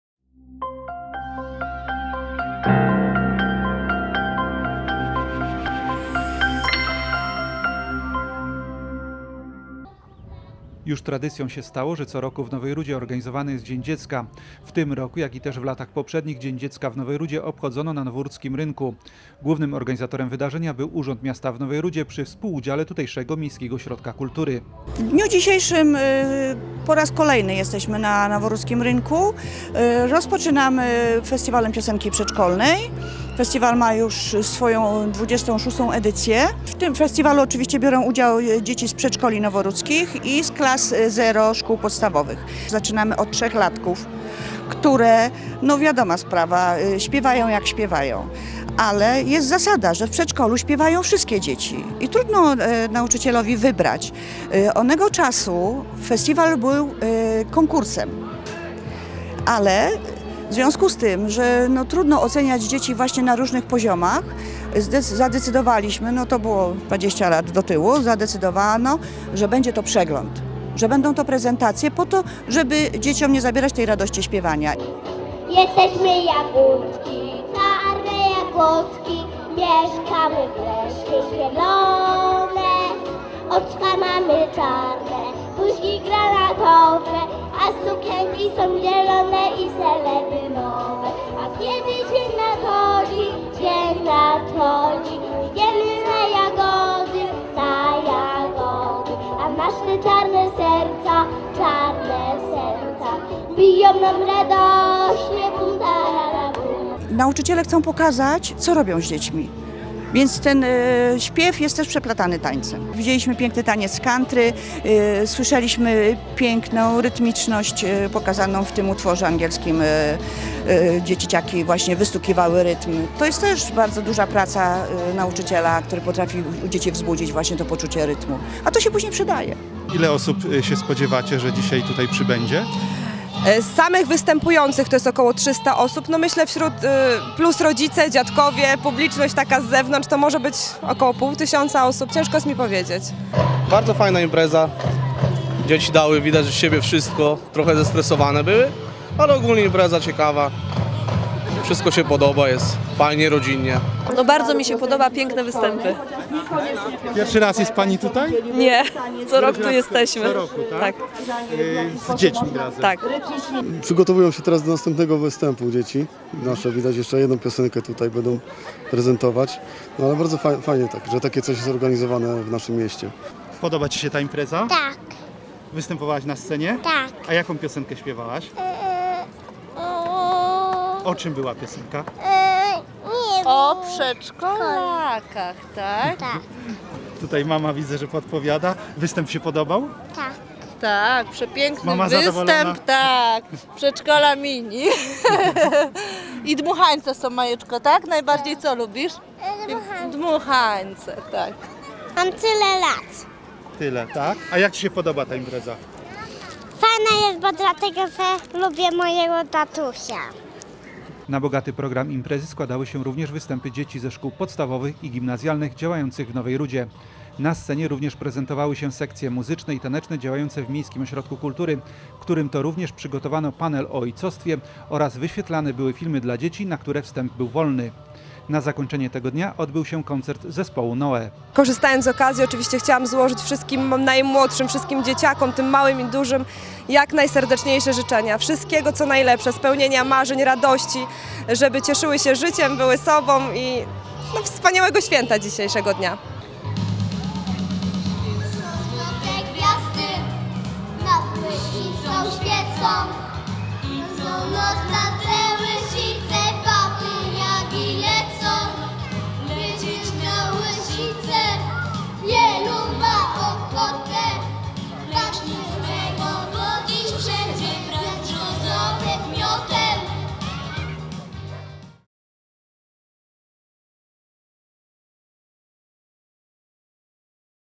Dzień Dziecka w Nowej Rudzie
Na noworudzkim rynku MOK w Nowej Rudzie zorganizował Dzień Dziecka.